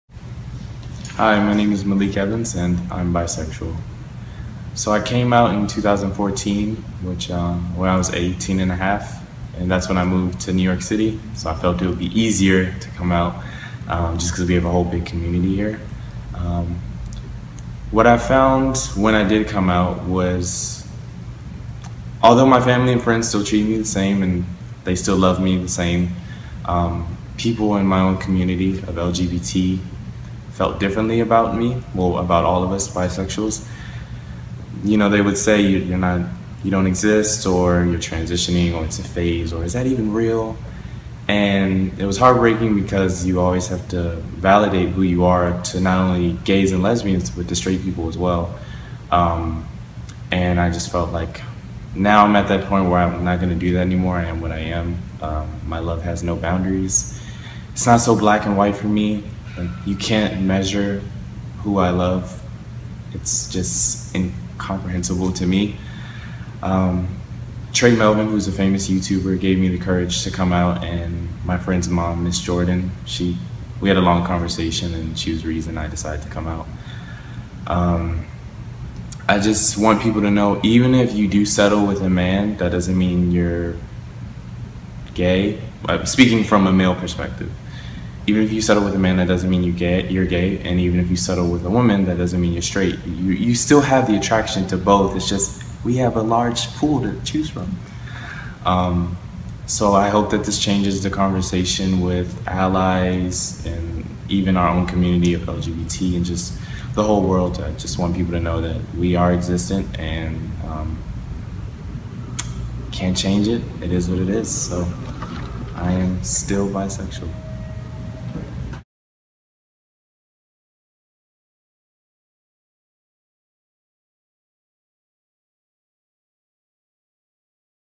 StillBisexual - Maalik Evans
StillBisexual is a social media and video campaign that aims at dispelling one of the main misconceptions about bisexuals — that they don’t stay that way.

Filmed at the NYC StillBisexual Campaign Day